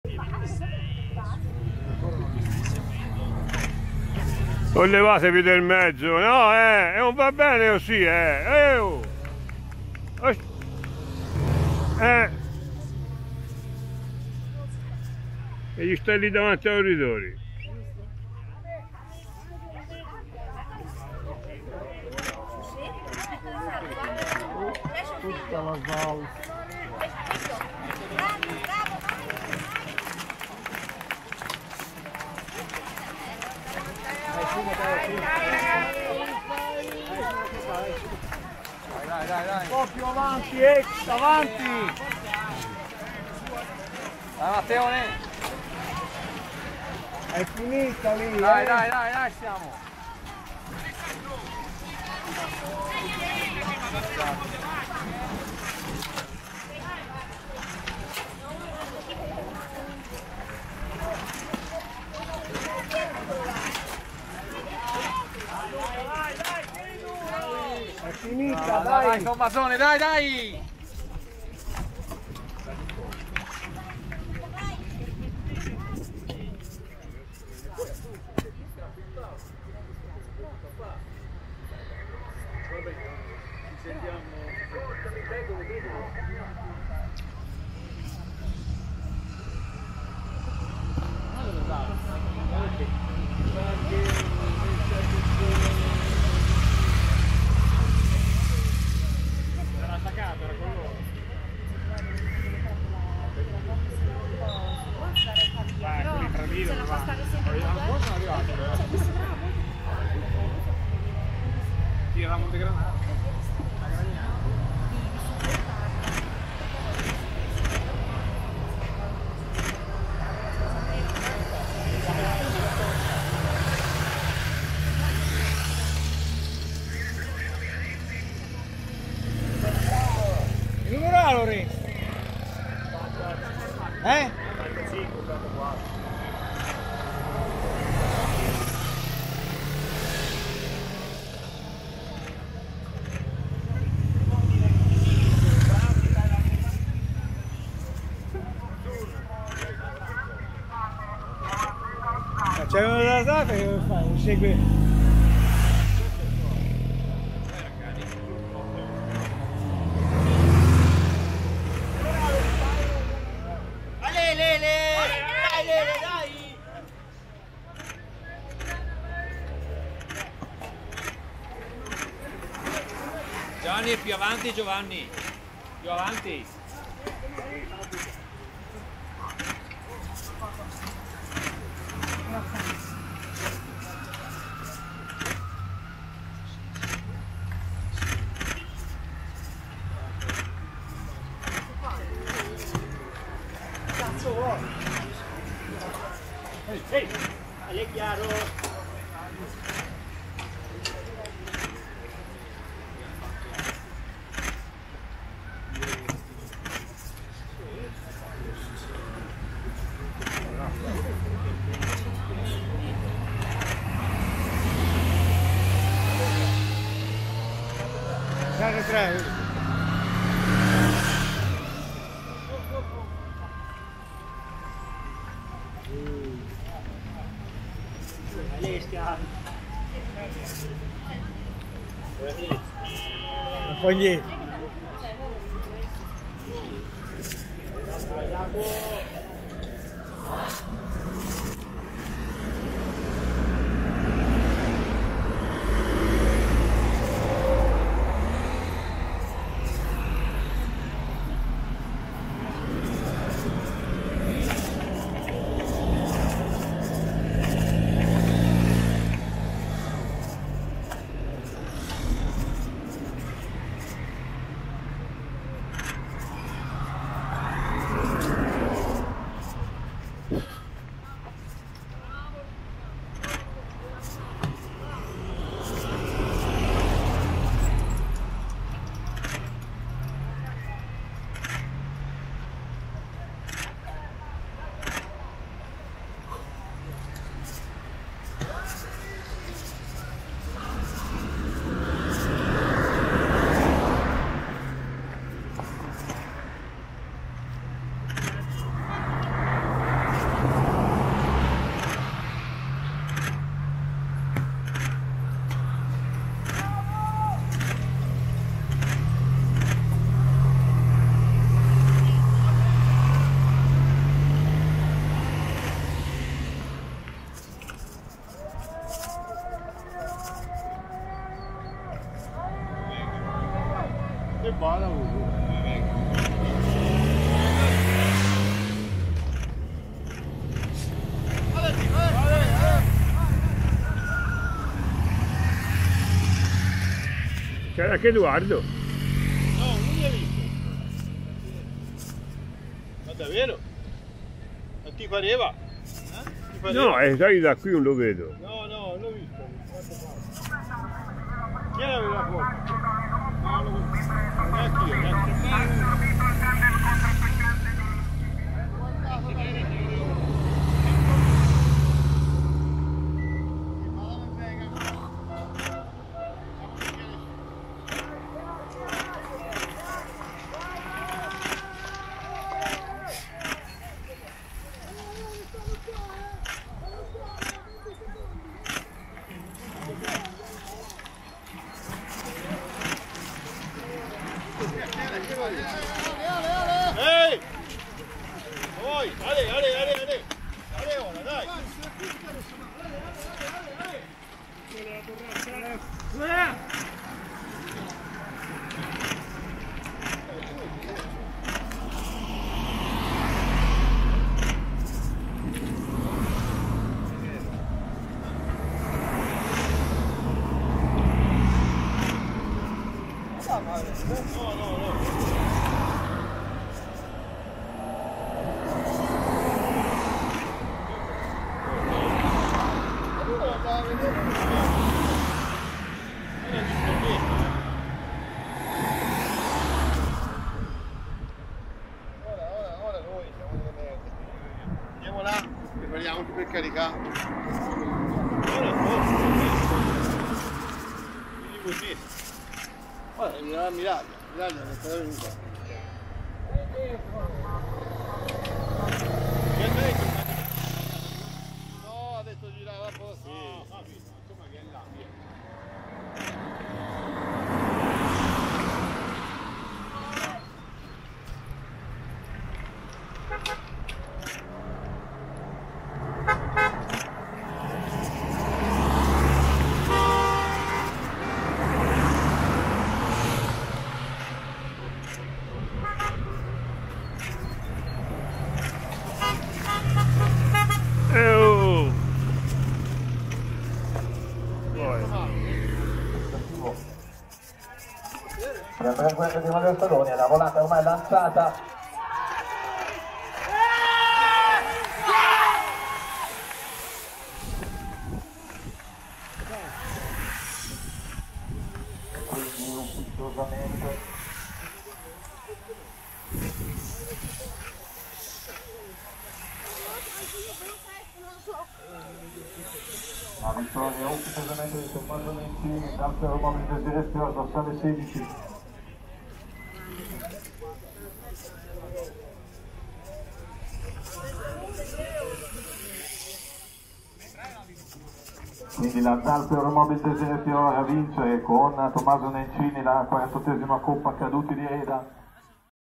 Coppa Caduti di Reda 2024
Tommaso Nencini conquista la prima vittoria stagionale alla Zalf.
Pettinati Communication - Ufficio comunicazione e web marketing